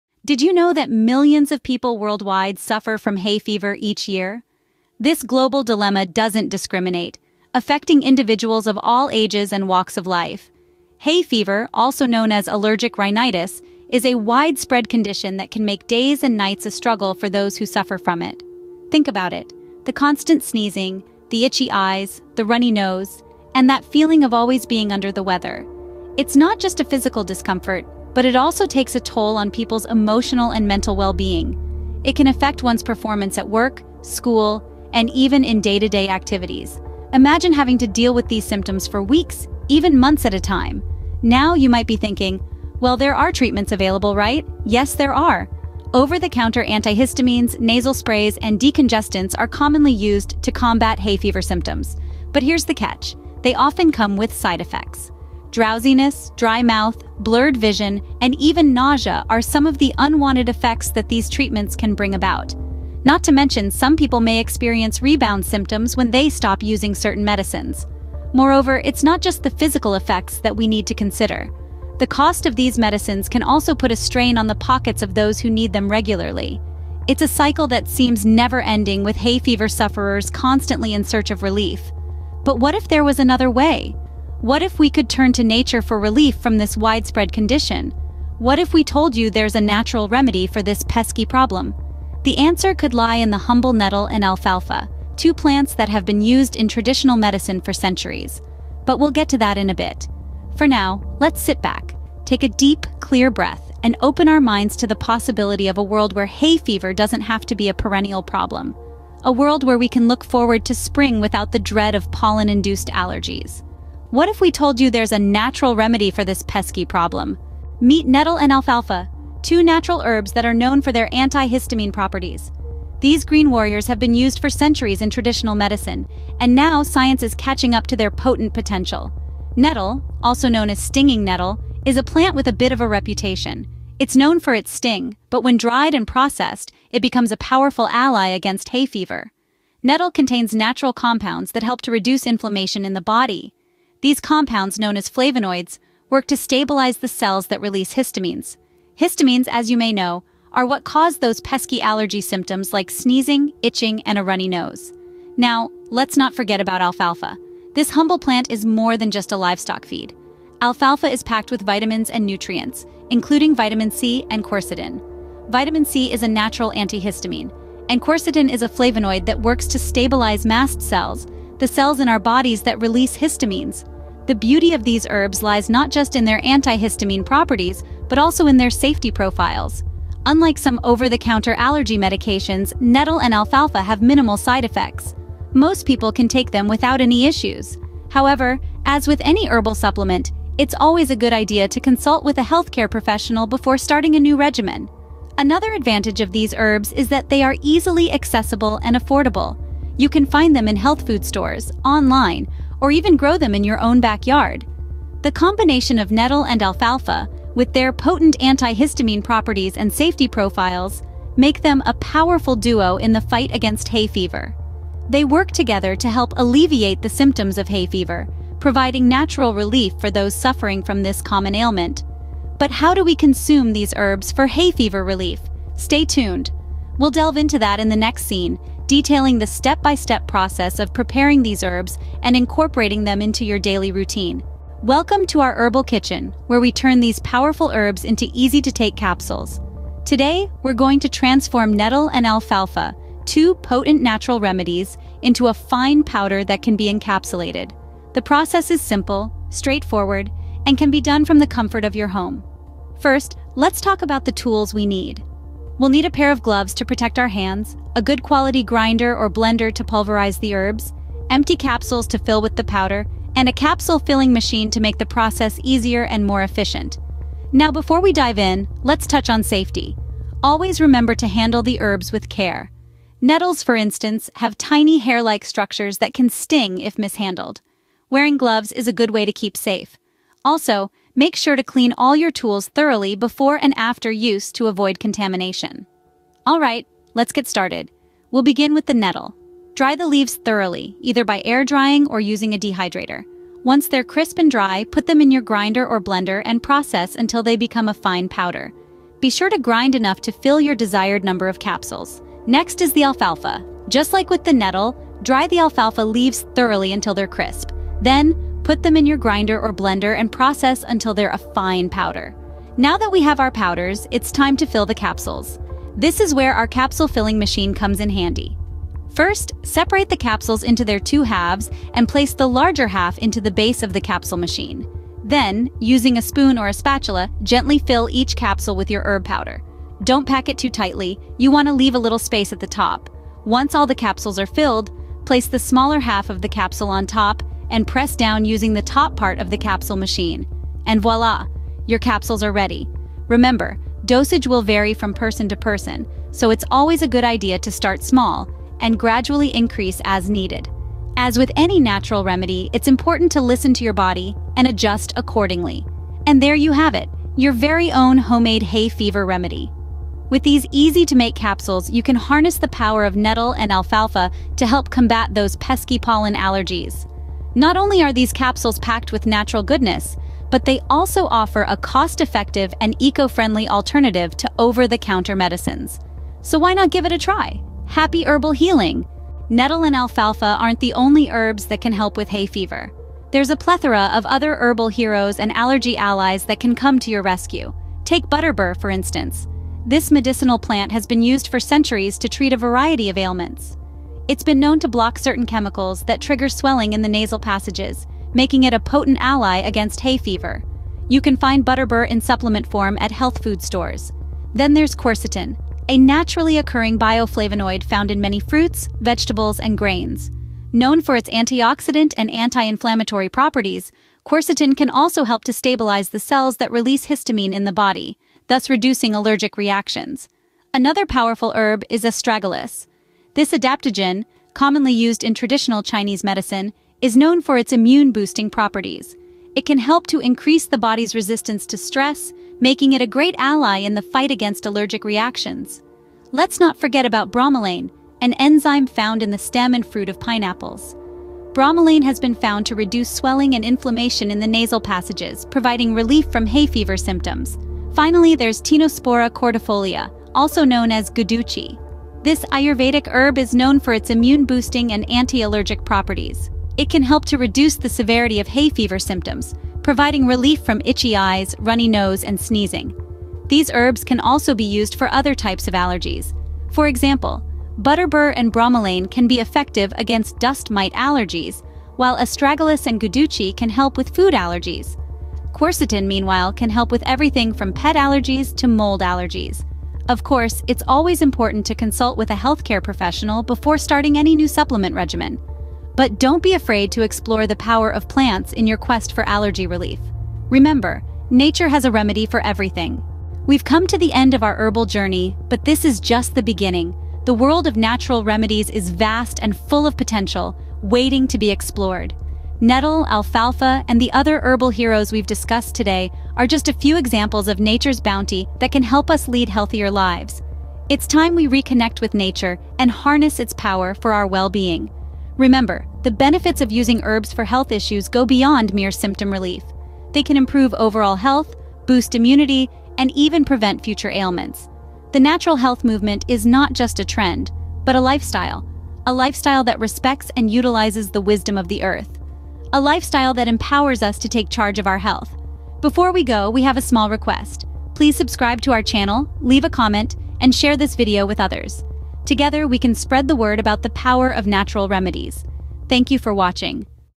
Nature's Cure: DIY Herbal Remedies for Pollen Allergy
Nature's Cure:  DIY Herbal Remedies for Pollen Allergy @JHealthylifestyle-cw2wy
This video is about how to cure pollen allergy with herbs using nettle and alfalfa.
It also gave brief talk of some herbs used to cure food and other allergies using different herbs. watch it to discover many things.